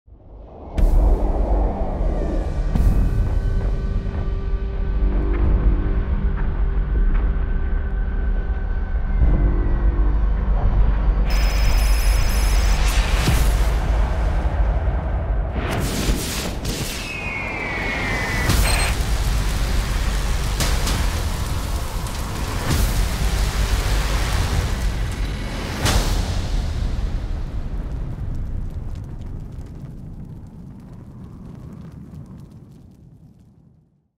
Scorpius Missiles Fill the Sky – Warhammer: The Horus Heresy
Get ready to rain down fire on the Loyalists/Traitors (delete as appropriate) with the Scorpius – get a closer look at the new plastic kit